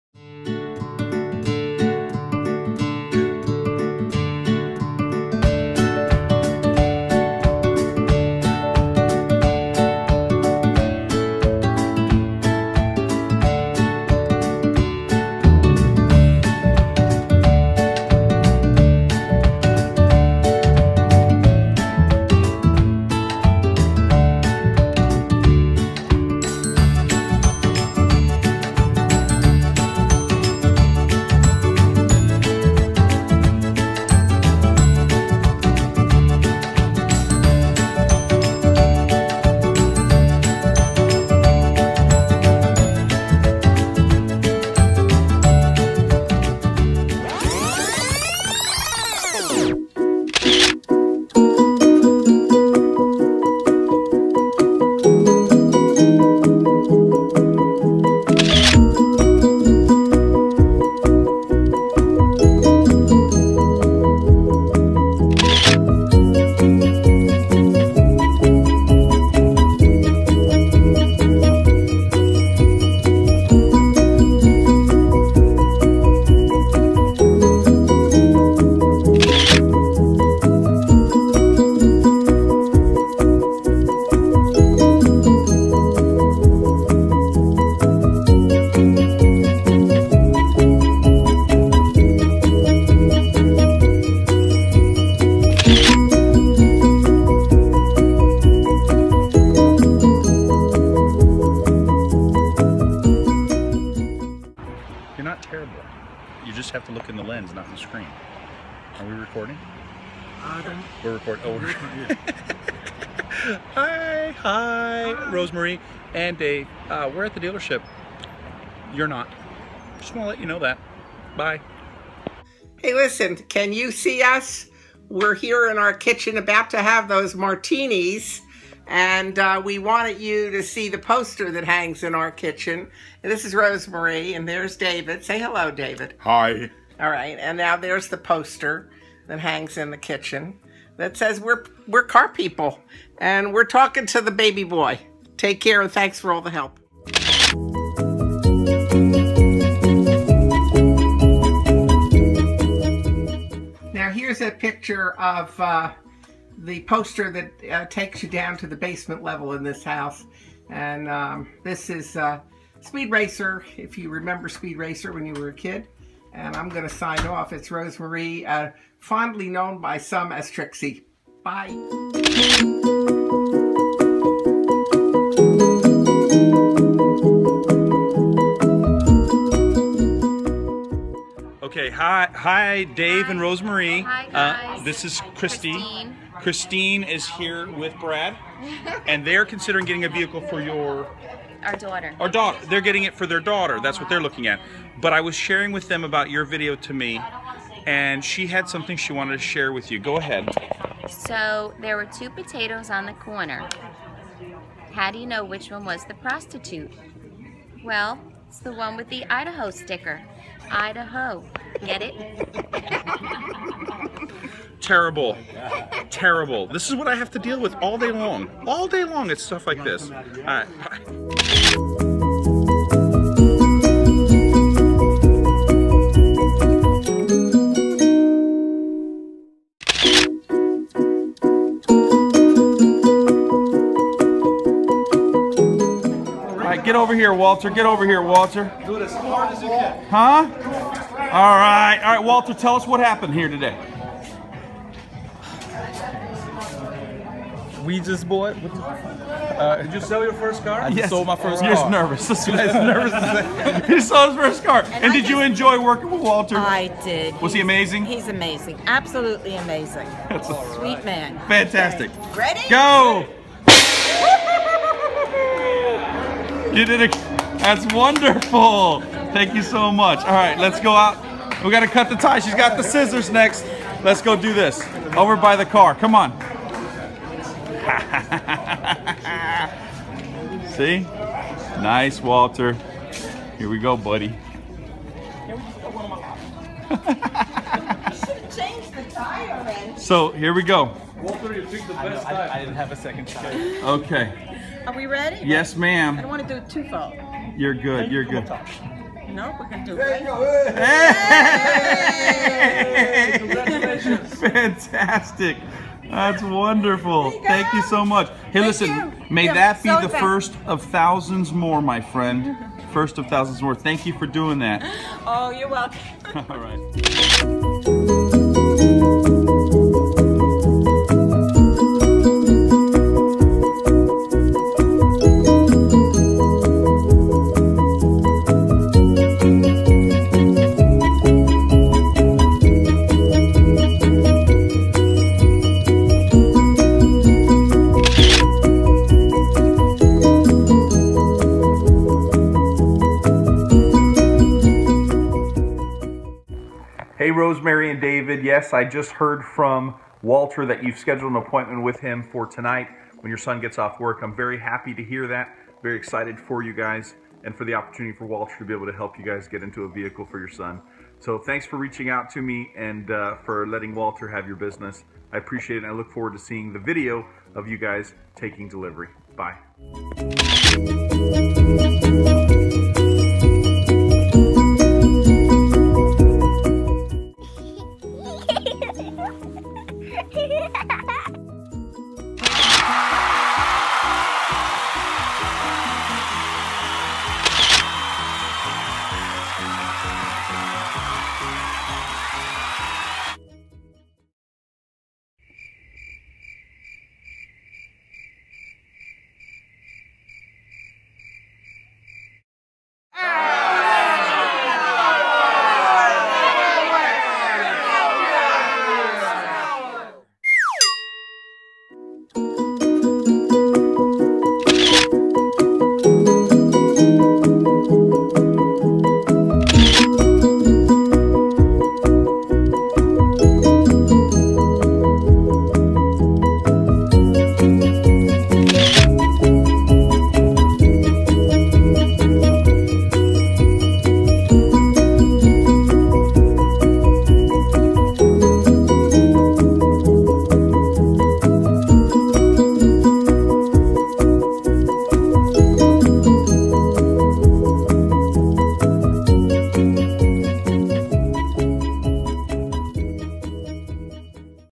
Sellchology - Most Adorable Customers - My deal with Walter
Join Jonathan Dawson of Sellchology sales training as he follows up with a sale for 2 months to get a sale for a salesperson.

Jonathan teaches the psychology of sales but he also practices what he preaches! Watch this video of the most adorable customers Jonathan has helped lately.

Enjoy this fun video for automotive sales follow up!